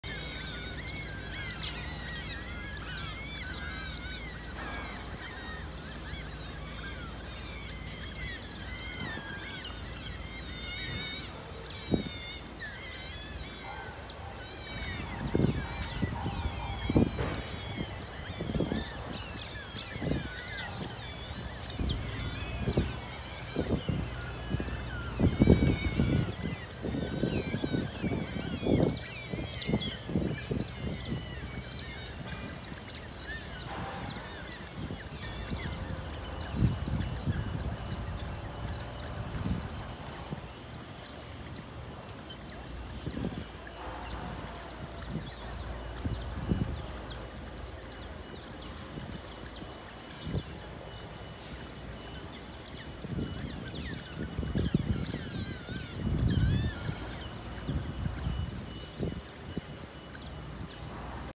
Wild Otter Family in Singapore
Found this large otter family on one of my morning walks, thought its pretty rare and wanted to share it with you guys.